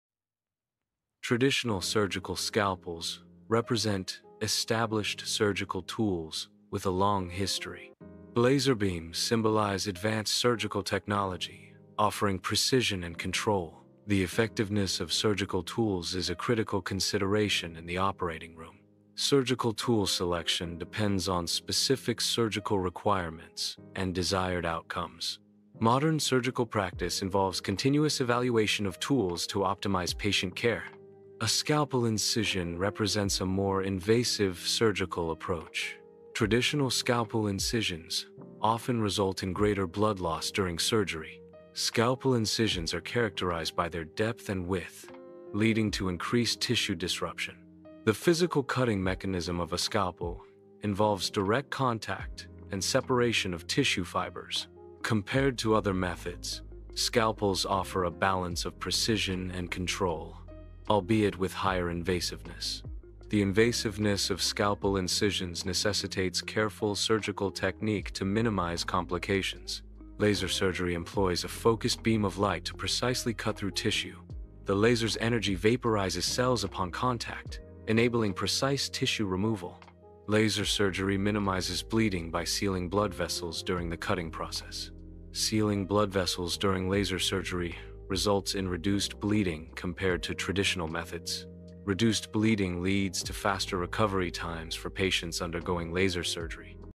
Scalpel or Laser? The Truth in 59 Seconds#Surgery
In under a minute, we're settling the ultimate debate: scalpel or laser? This short video breaks down the pros and cons of traditional vs. laser surgery. Discover which tool offers greater precision, a faster recovery, and why the choice depends on the procedure.
Did you know the truth about these surgical tools?